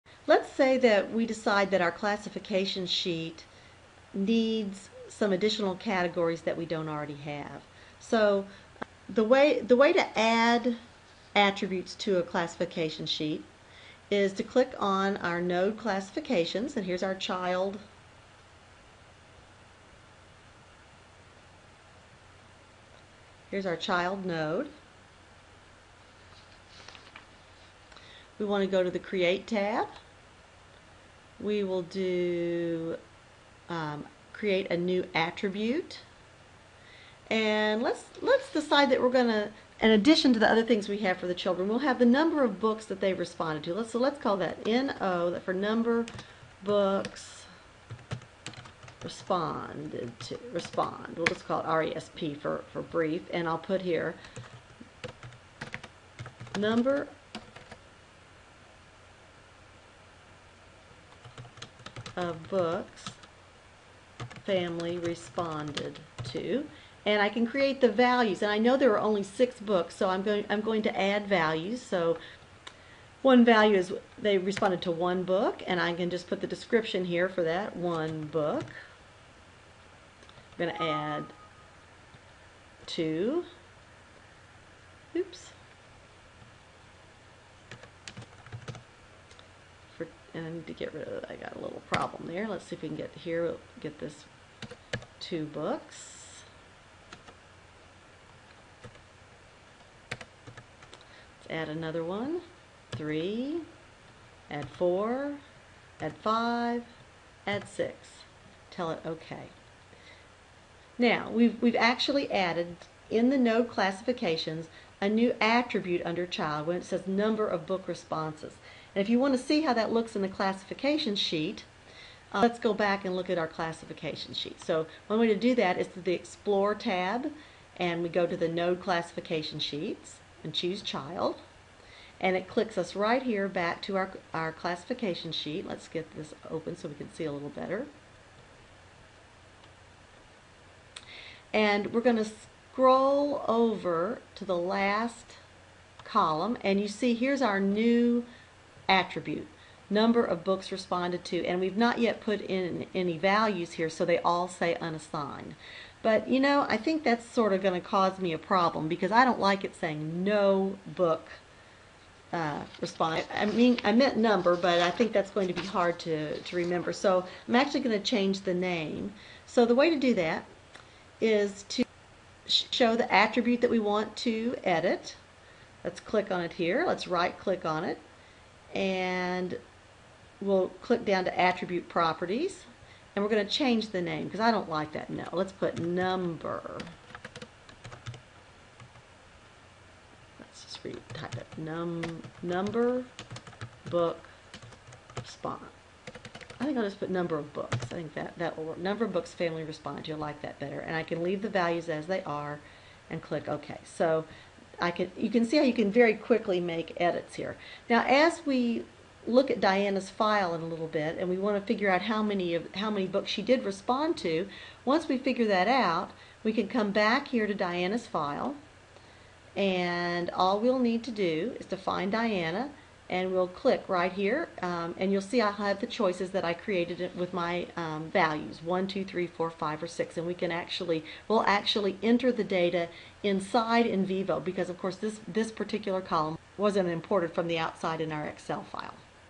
NVivo 10: Coding Sources to Classification Nodes
This NVivo10  demonstration shows how to add and edit attributes and values in an existing classification sheet.  It shows how to code existing data sources to node classifications so that data sources are linked to participants' case nodes and their demographic characteristics.